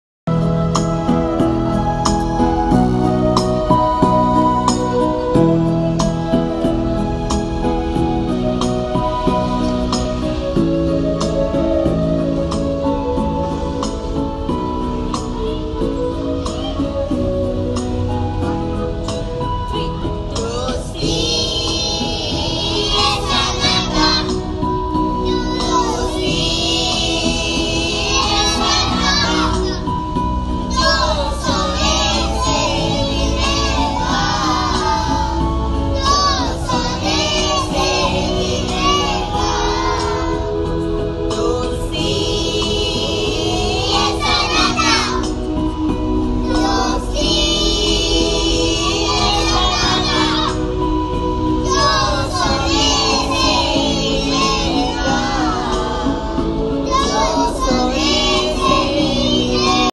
Students of North Chapter paying tribute to a legendary musician Sohail Rana
Students of North Chapter paying tribute to a legendary musician Sohail Rana who worked passionately for the children of beloved Pakistan.